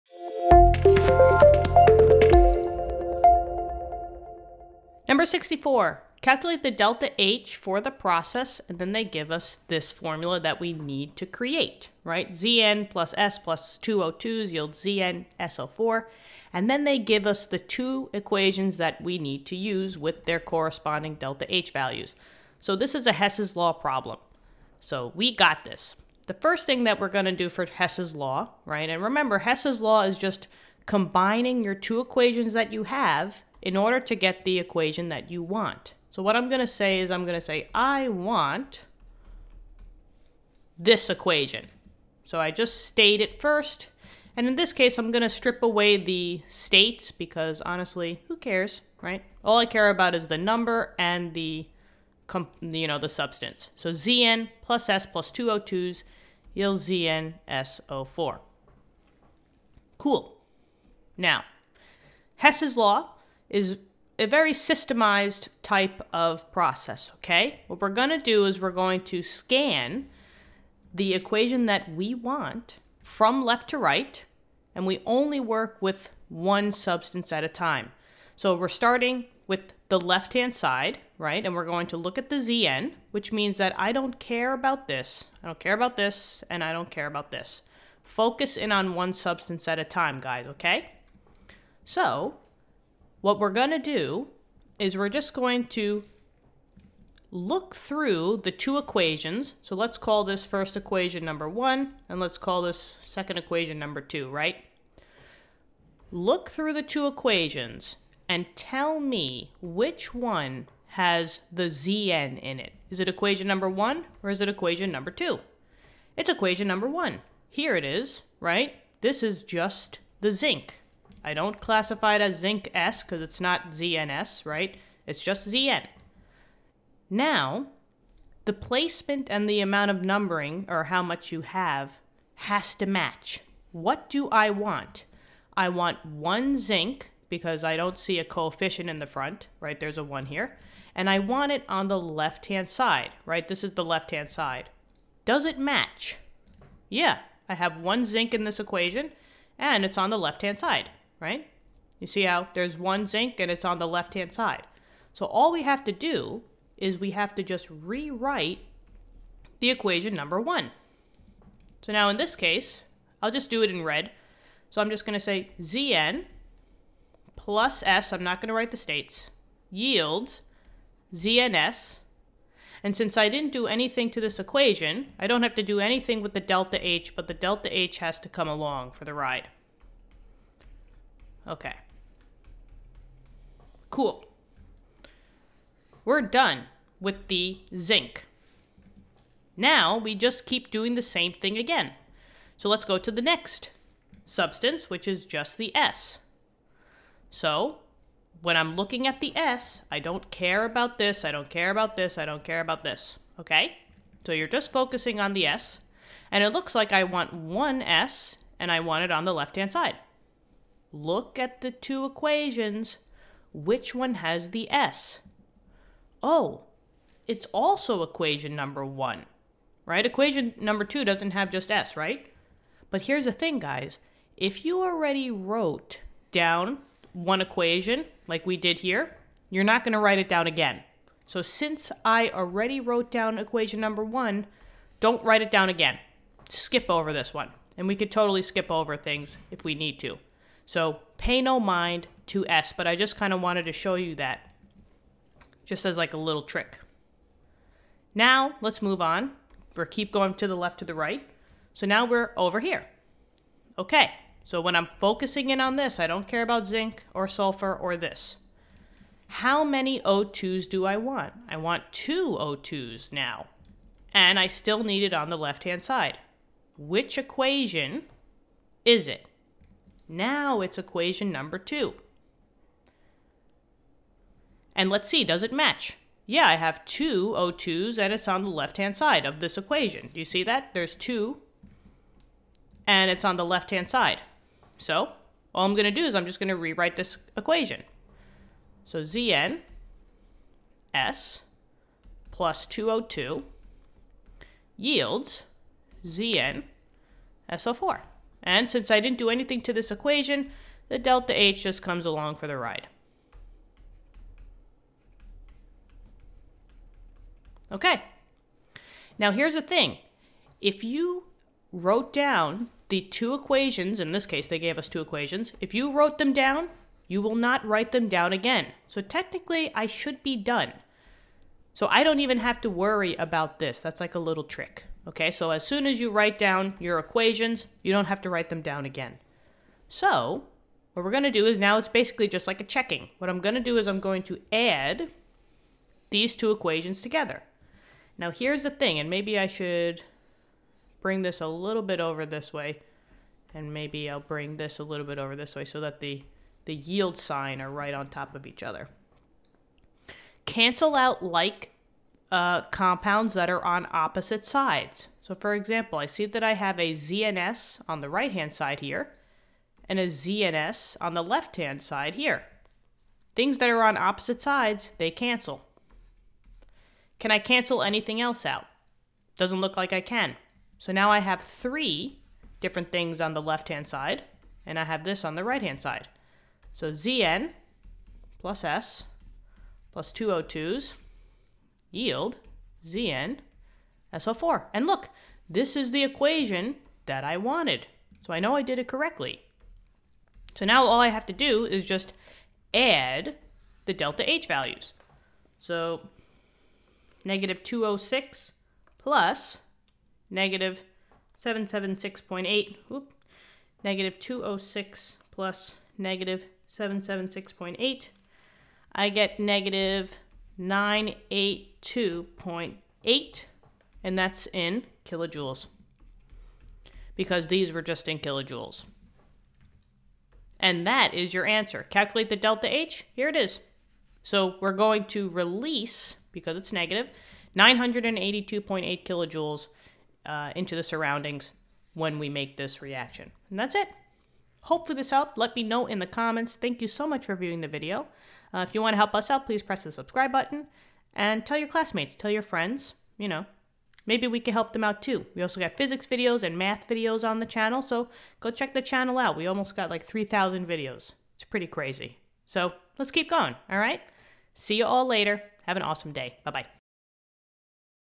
5.64 | Calculate ΔH° for the process Zn(s) + S(s) + 2O2(g) → ZnSO4(s) from the following information
Calculate ΔH° for the process Zn(s) + S(s) + 2O2(g) → ZnSO4(s) from the following information:
Zn(s) + S(s) → ZnS(s) ΔH° = −206.0 kJ
ZnS(s) + 2O2(g) → ZnSO4(s) ΔH° = −776.8 kJ